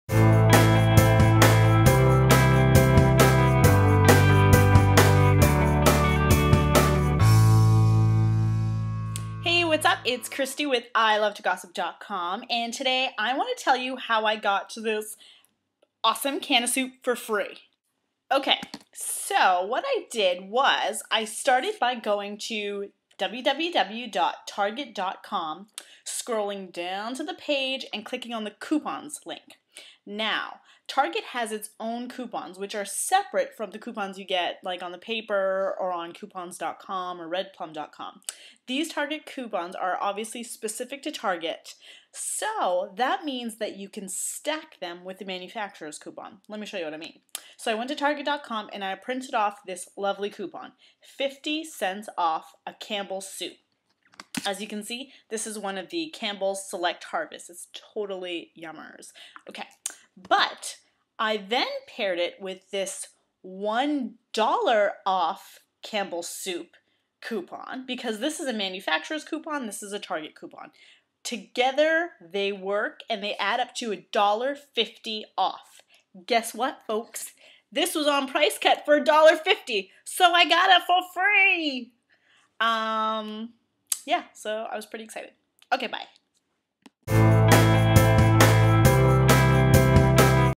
How to use Target Coupons to get FREE Stuff!
In this video, CC from ilovetogossip.com shows you how to stack Target coupons with manufacturer's coupons for even bigger savings!